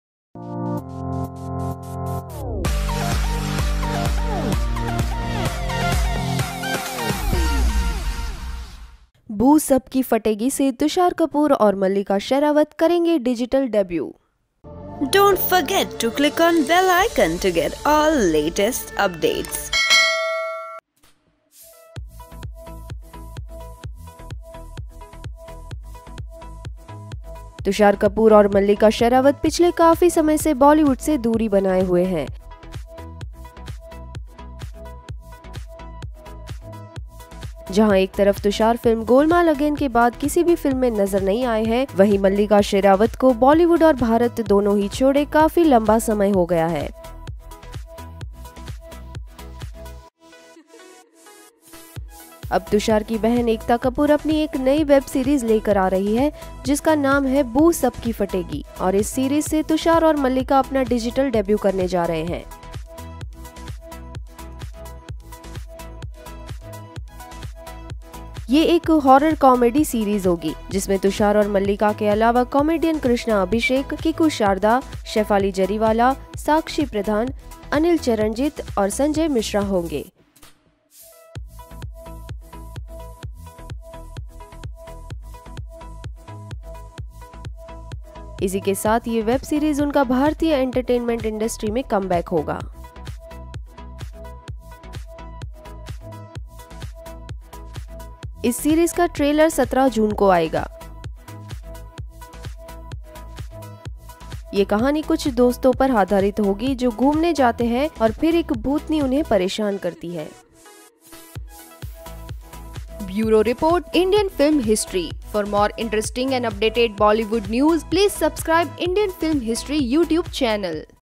Mallika Sherawat, Tusshar Kapoor 'बू सबकी फटेगी' से करेंगे डिजिटल डेब्यू
Address:
1/3,Shere Punjab Colony,
Andheri East.
Mumbai 400 093
India.